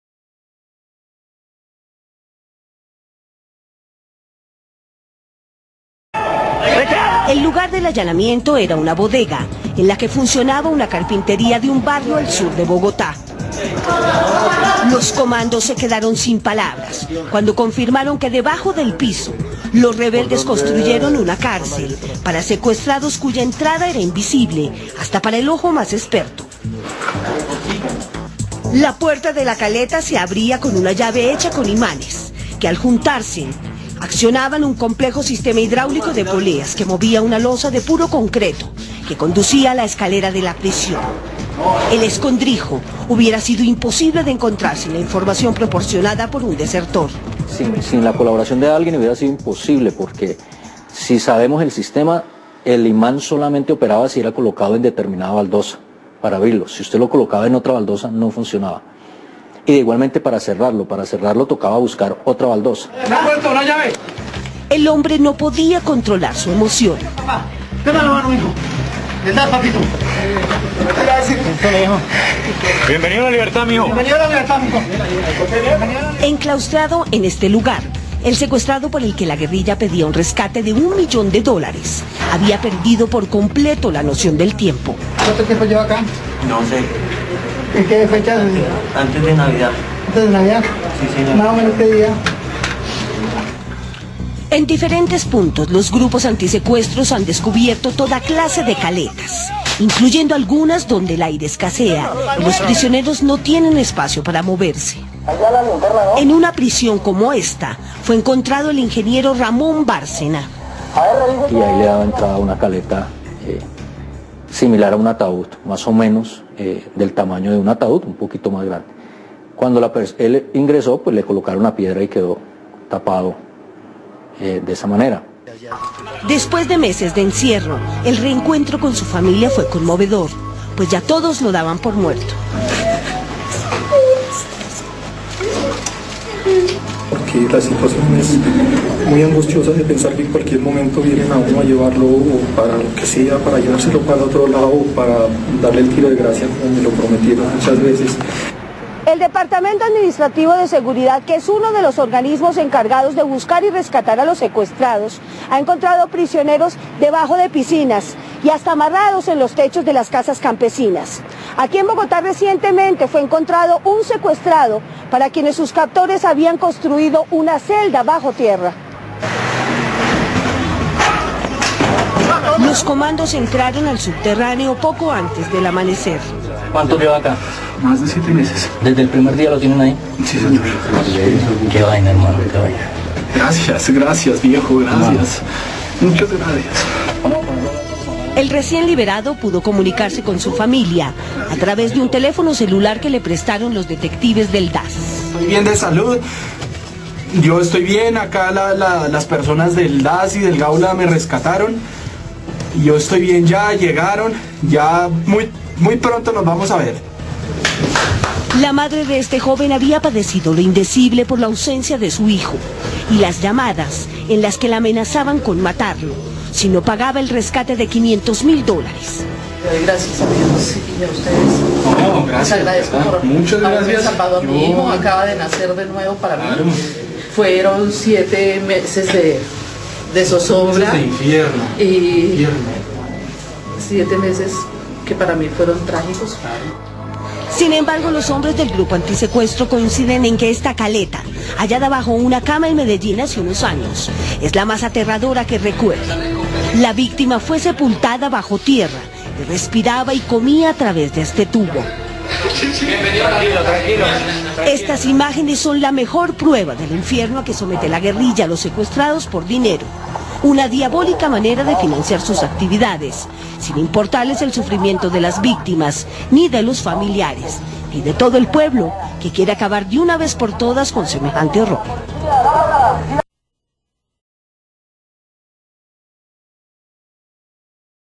COLOMBIA: Descubren cámaras de torturas subterráneas de las FARC
Ya han liberado las FARC a seis prisioneros según: EL ESPECTADOR, ya solo faltan 2536 rehenes colombianos Yo no vivo en colombia pero es triste ver como esas personas hacen dañoy esos son los amigos de Chávez el tirano, los esta armando, ayuda militarmente, ofrece territorio venezolano como santuario y pare de contar...